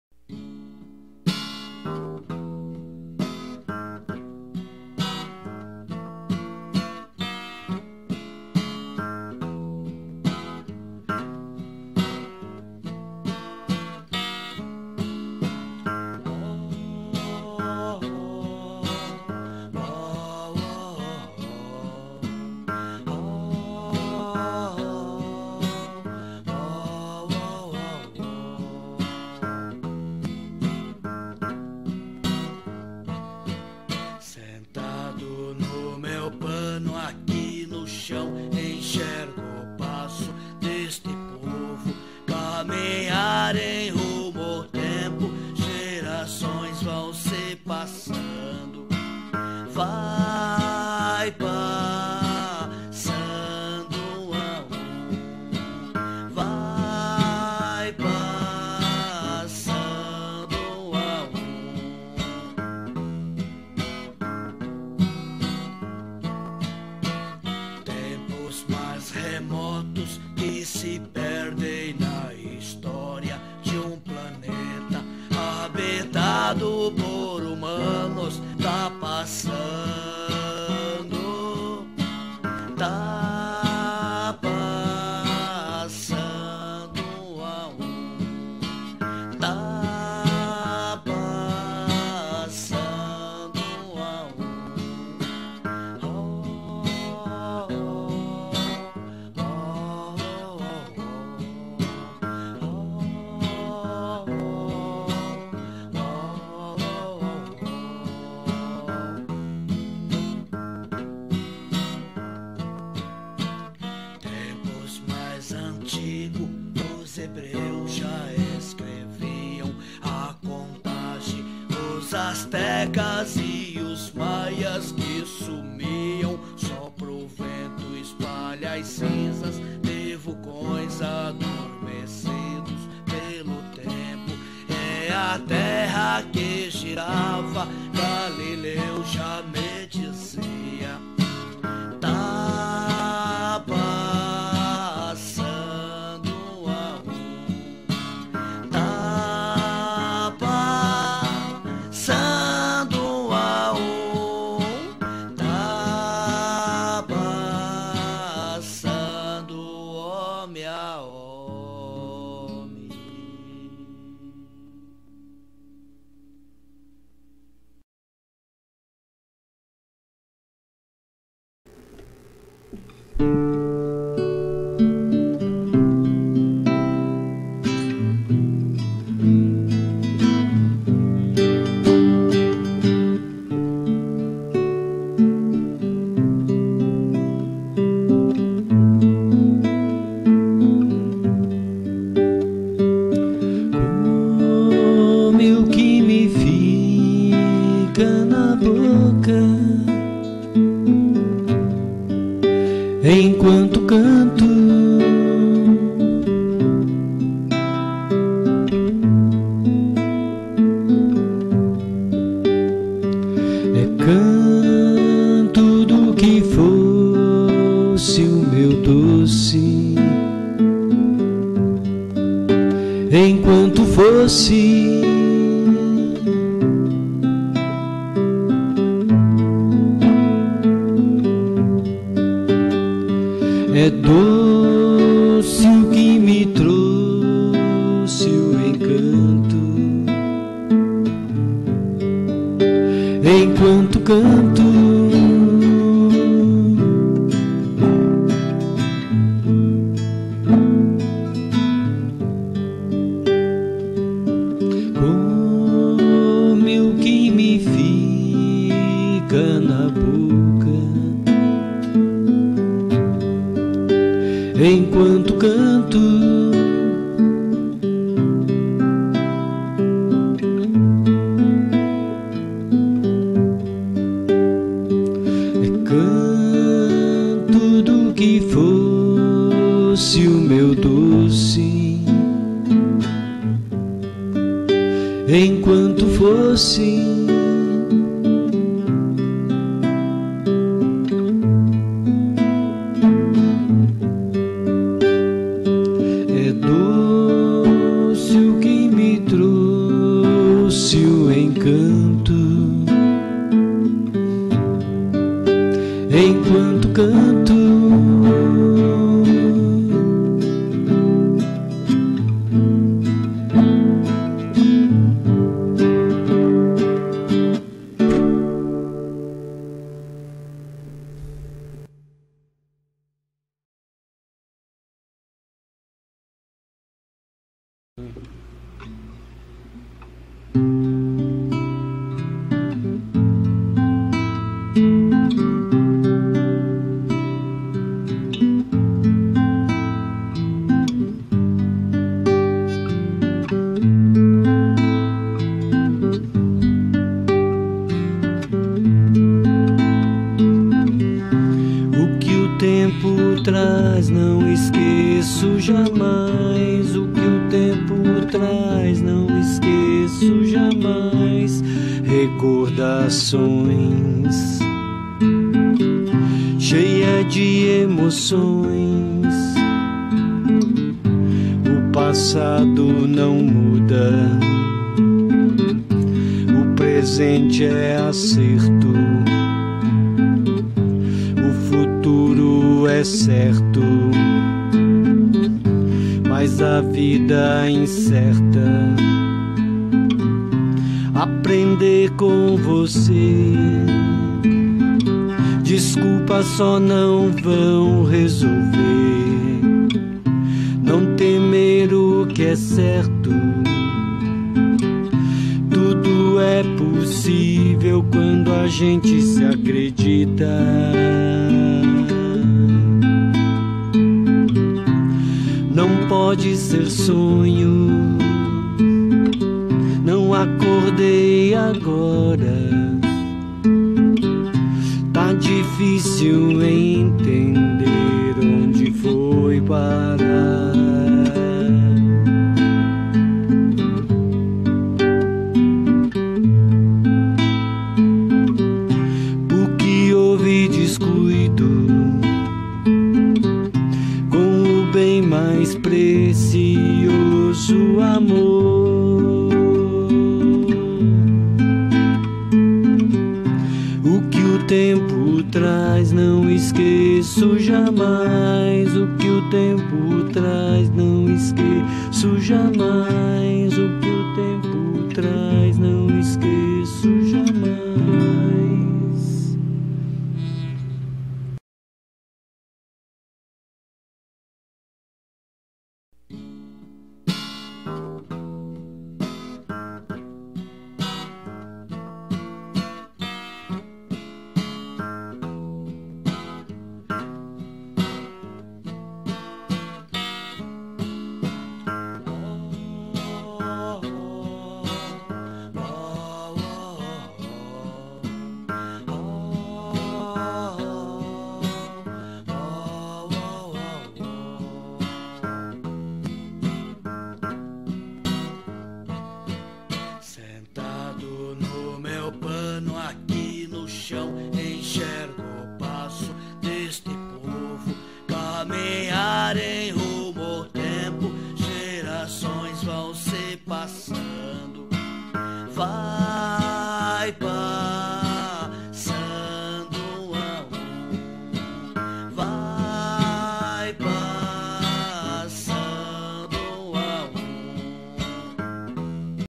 Aprendendo e Ensinando em Brasília
Filme do Prof. René Scholzna sua ida a Brasília participar da etapa final do Concurso Aprender e Ensinar Tecnologia Social